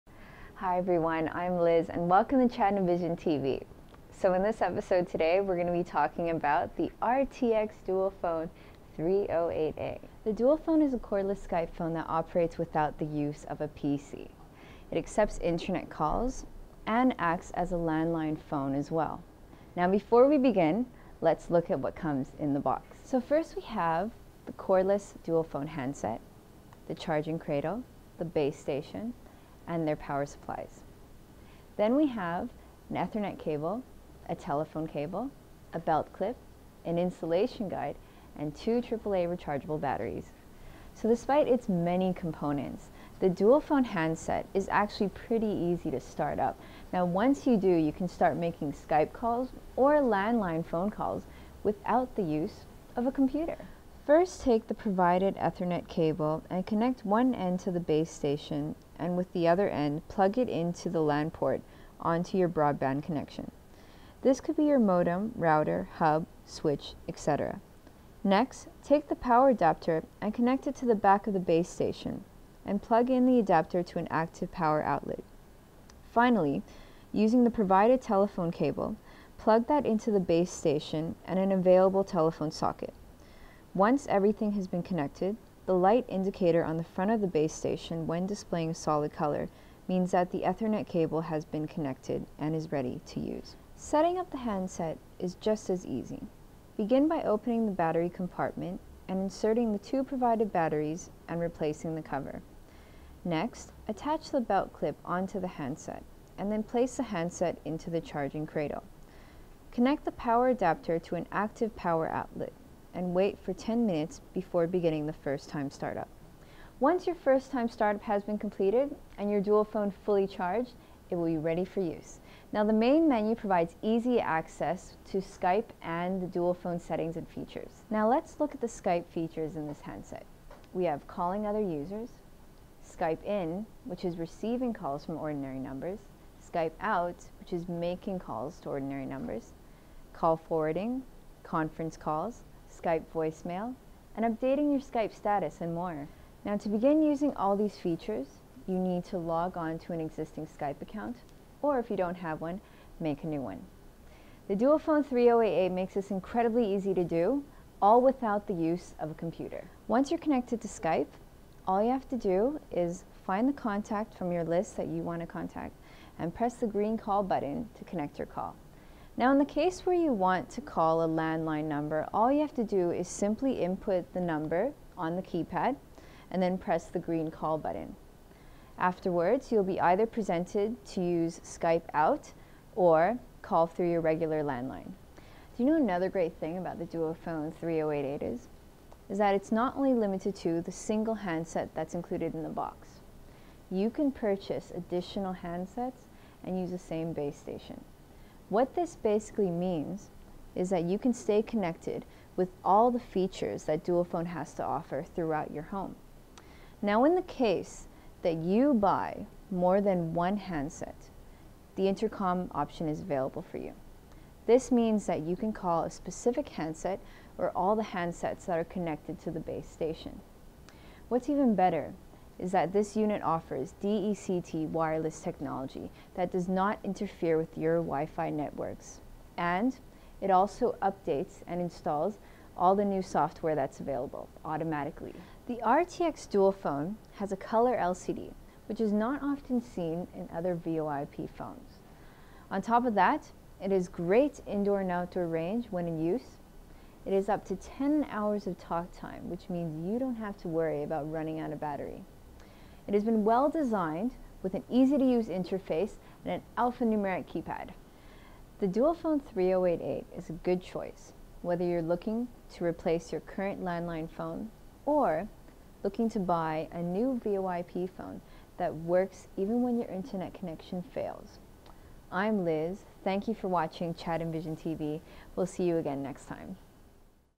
RTX Dualphone 3088 Review
The RTX Dualphone 3088 is a cordless Skype phone that operates without the use of a PC and supports not only internet (VOIP) calls but landline phone calls as well.